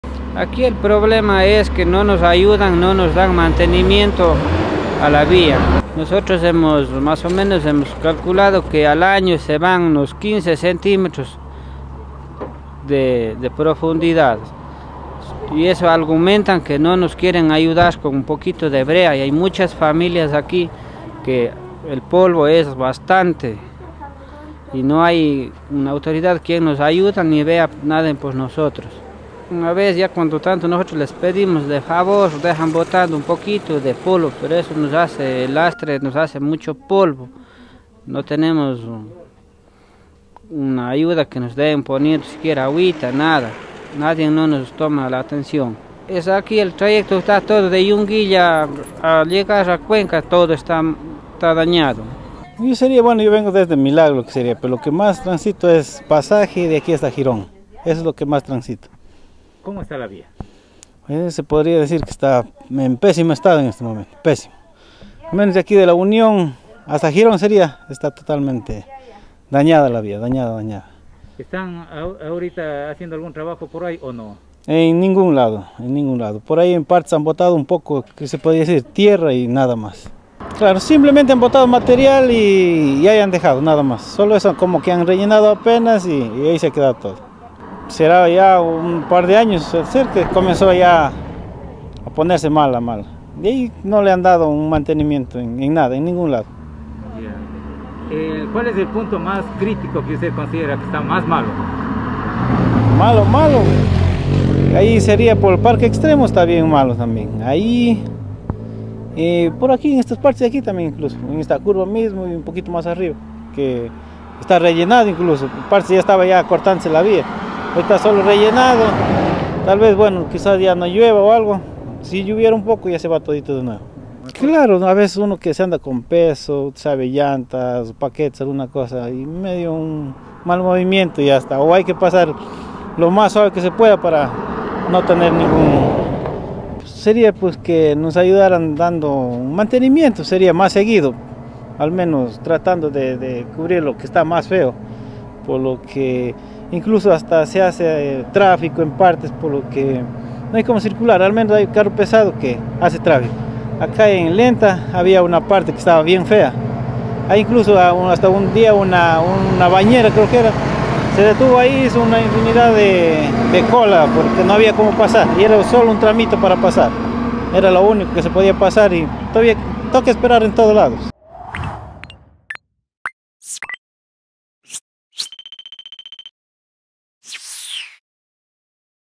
La vía estatal en la jurisdicción de Girón continúa en deplorables condiciones
Pocos días duró la presencia de maquinaria en los distintos puntos críticos en la vía estatal jurisdicción de Girón aseguran habitantes de sectores colindantes con la carretera, se hicieron parches emergentes, pero con el pasar de los días el descuido ha vuelto a la normalidad. Un recorrido realizado por este medio el pasado jueves constató que no había maquinaria laborando en la jurisdicción de Girón.